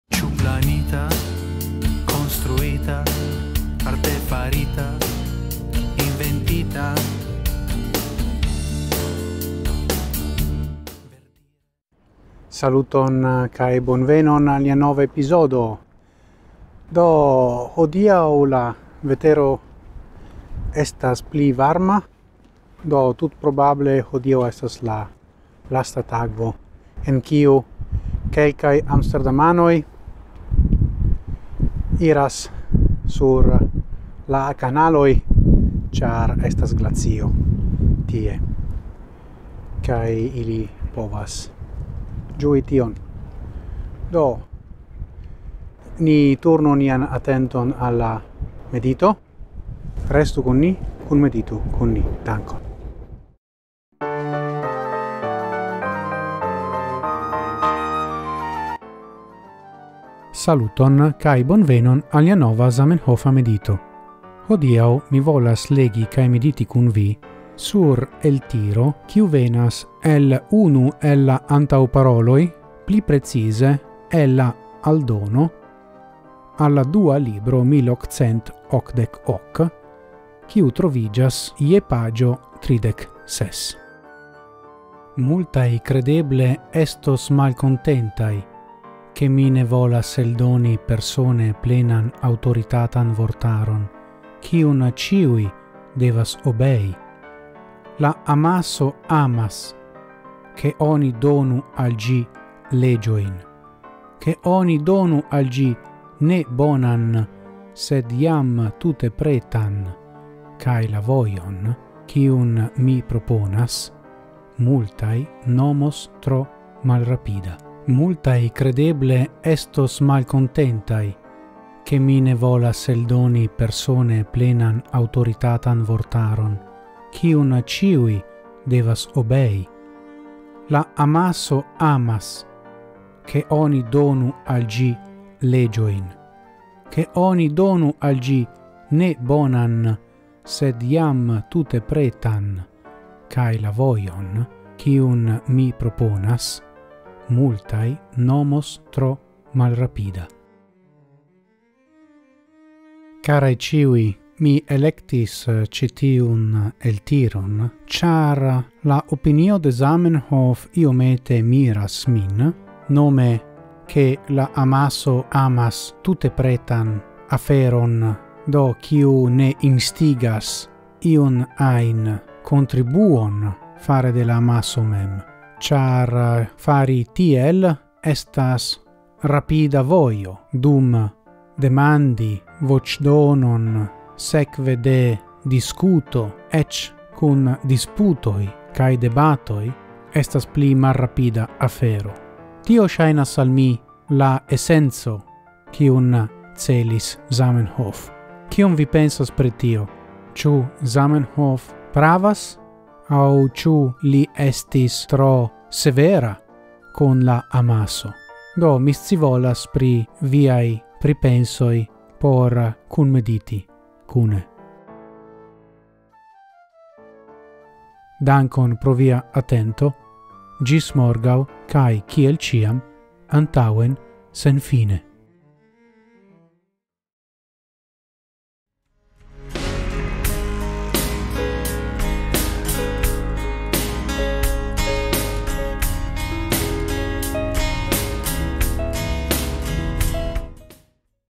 Zamenhofa Medito 273 + Antaŭmedito 123
Kion pensis Zamenhof pri la amaso? Kaj vi? Ĉu vi opinias same?